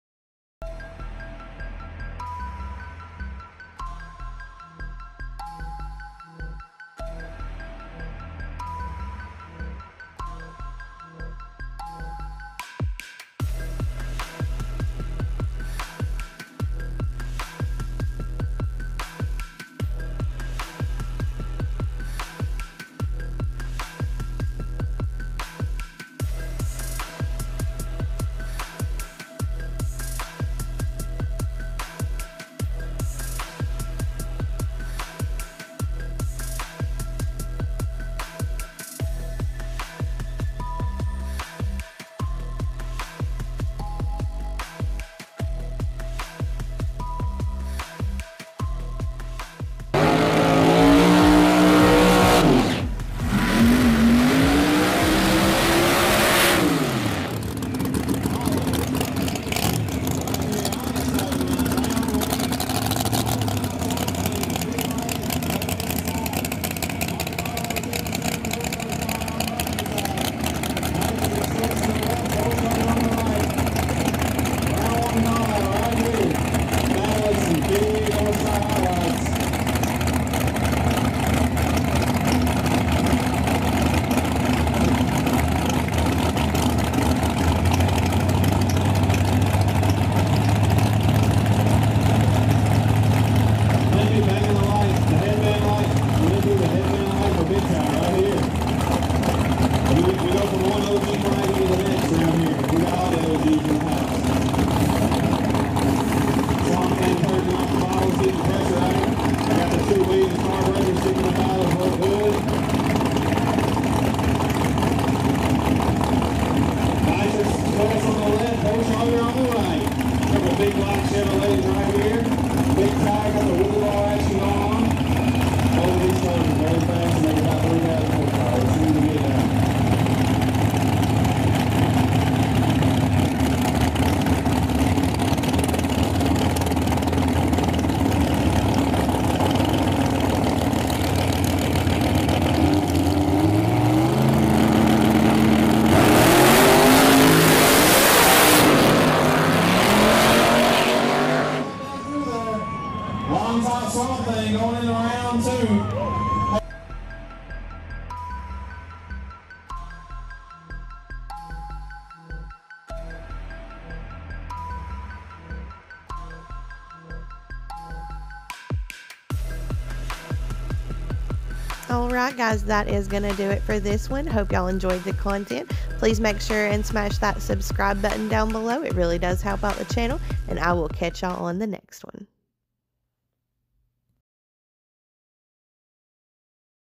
Battle On The Asphalt SWAMP THING 2K CALL OUT 1ST PAIR DOWN THIS THING IS AN ANIMAL!!!
SWAMP THING VS SCREAMER 1ST PAIR DOWN BIG TIRE BATTLE ON THE ASPHALT 2K CALL OUT